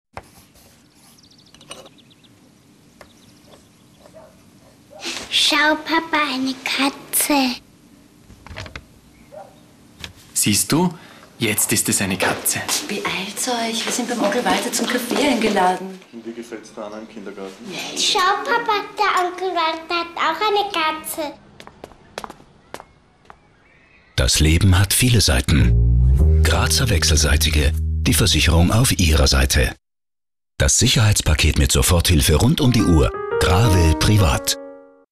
Schau Papa eine Katze!
Grazer Wechselseitige Versicherung Werbespot (Schau Papa der Onkel hat auch eine Katze...)